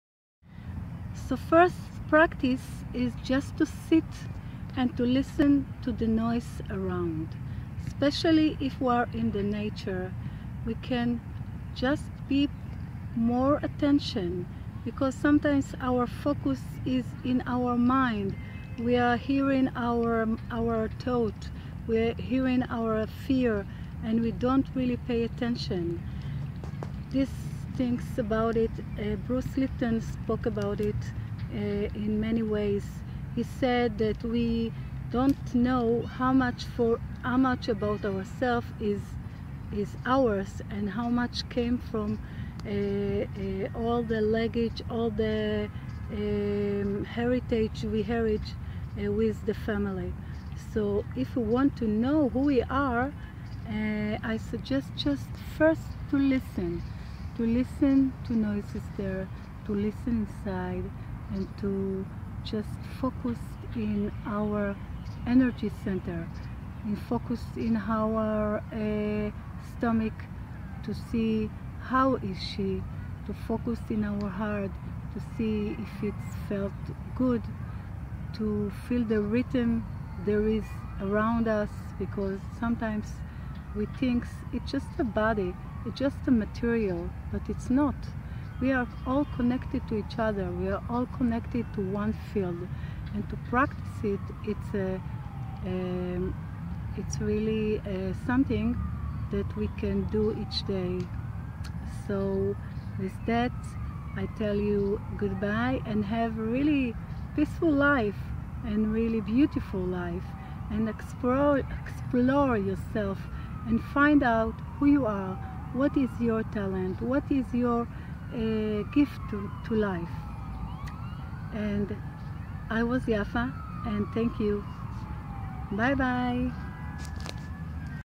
vlog 6-b - meditation and mindfulness #
meditation is a nessery exersis to a self healing, and improved relationship